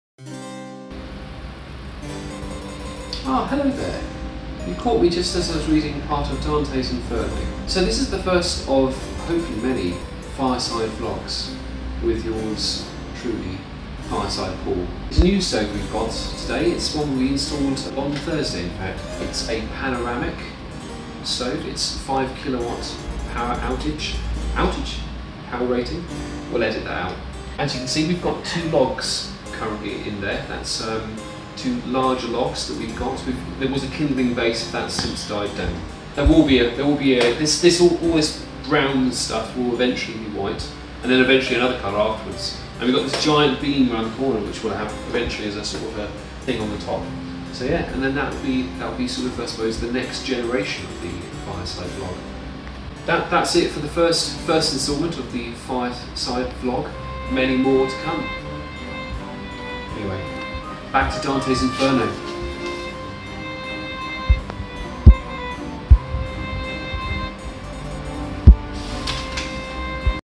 Fireside Vlog with Paul featuring Dante's Inferno
The first (and possibly last) in a new series of fire side chats with Camp Kevian Paul.